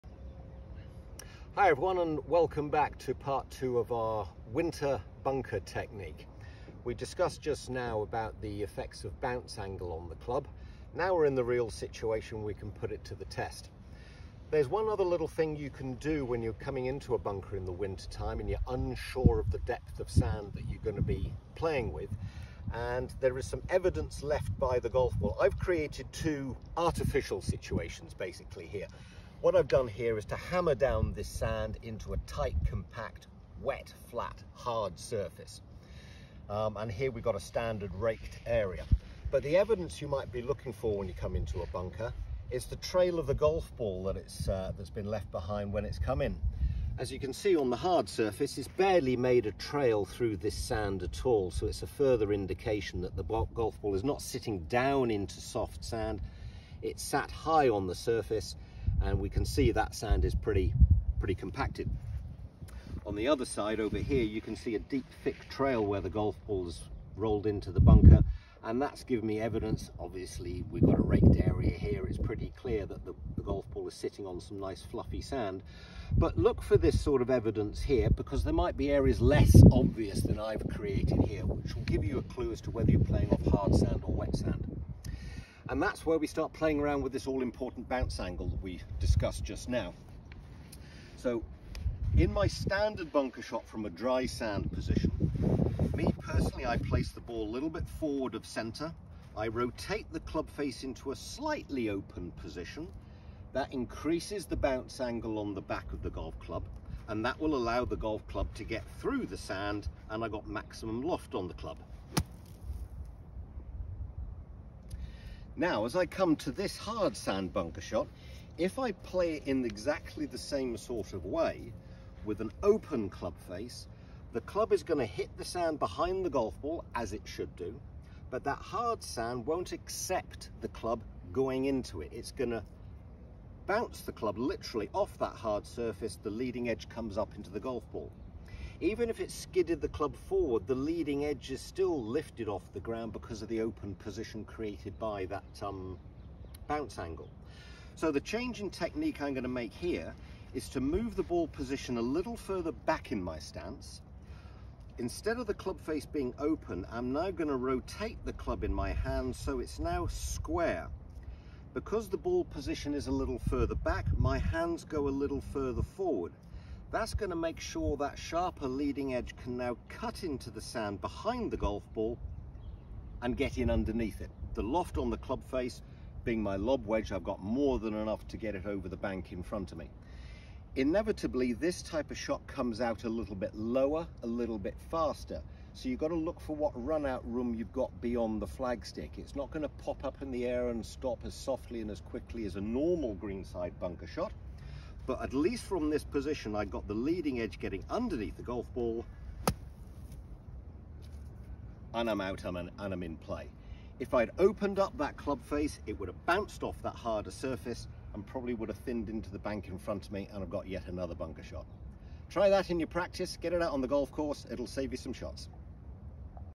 Winter Bunker Play Part 2
Struggling to get out of the bunkers?

Head professional at Coombe Wood Golf Club, Phil Wright goes through a winter bunker technique that will help you get on the green and putting. This time getting down into the bunker and showing us how it is done.

Try them out and let us know if there is anything more Phil can help you out with.